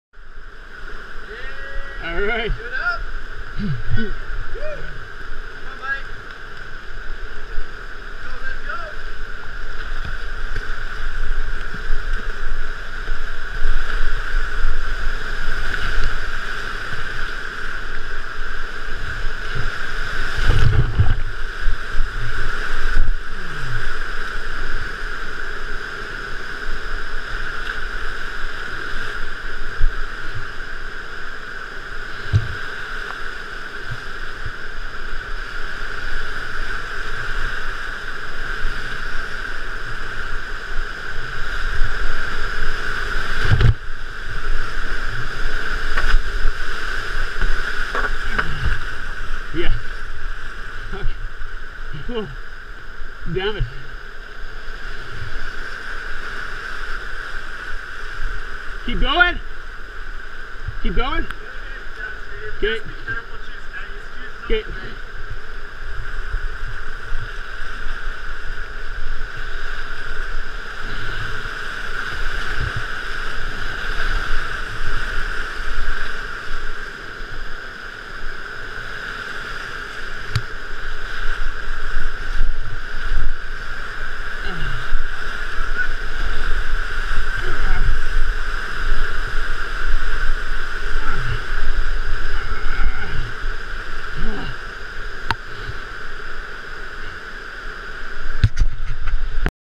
MIstaya canyon
MIstaya canyon run 2014 - WPP program with UAPS whitewater club